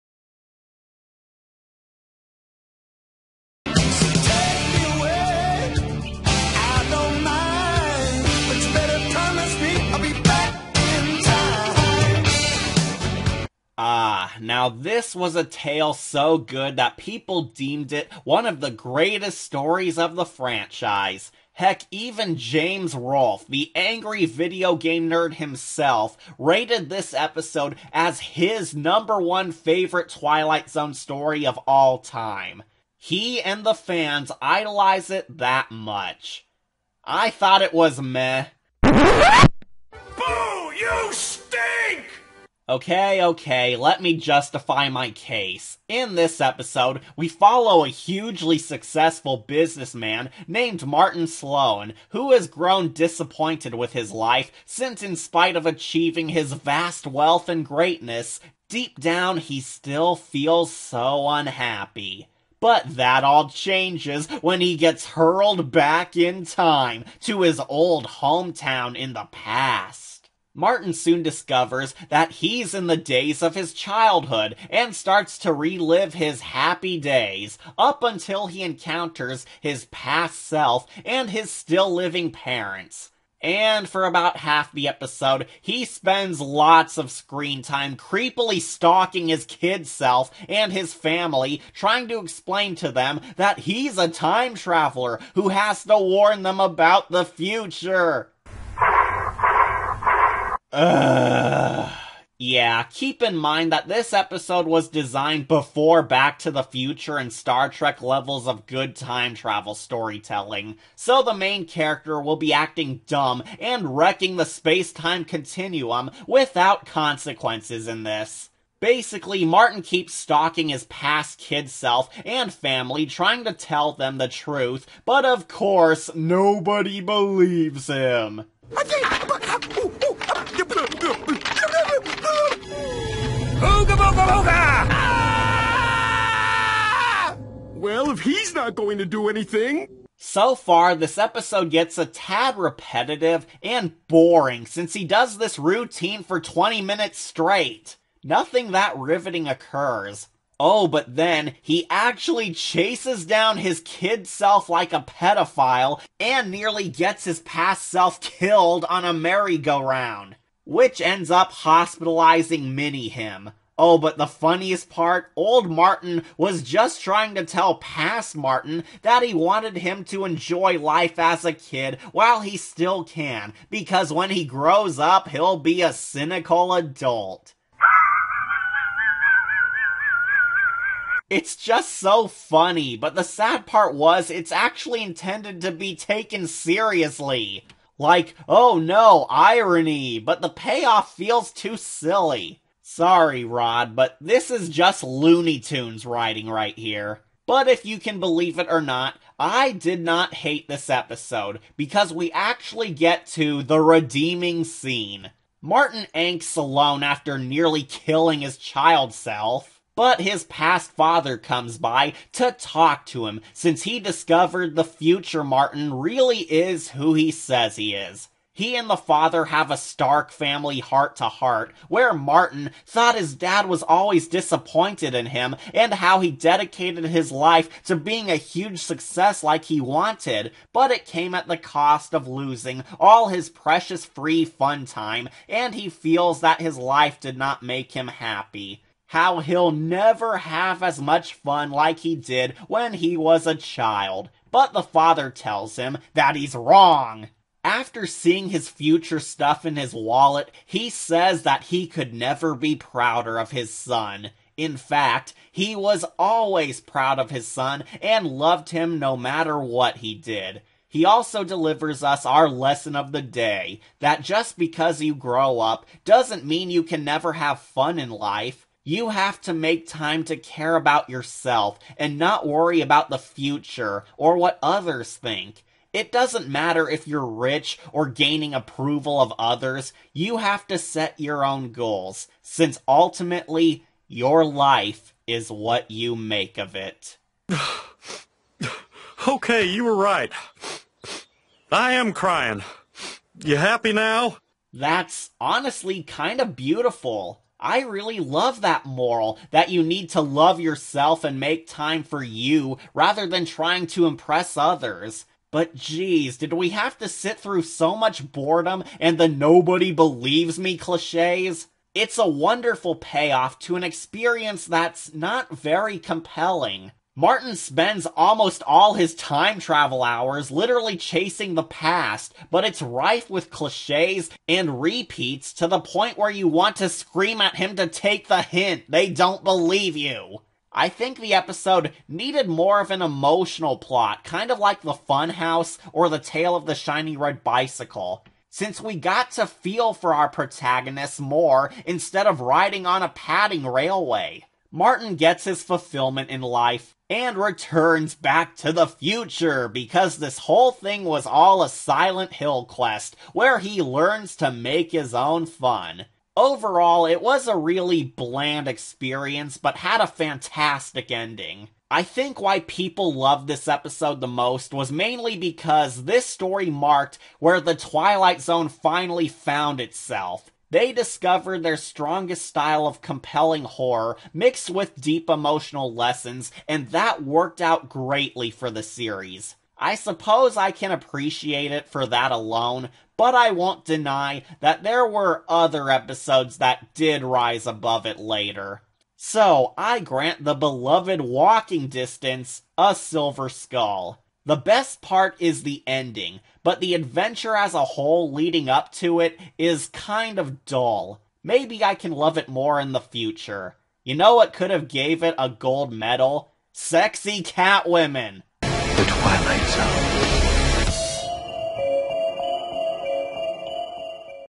Rapid-Fire In the Zones: Walking Distance
Cat takes a trip back in time and learns that the past is not as ideal as we remembered.

The Twilight Zone is owned by CBS and NBC.

Patrons (last names will be initial only if it seems like an actual name, sorry)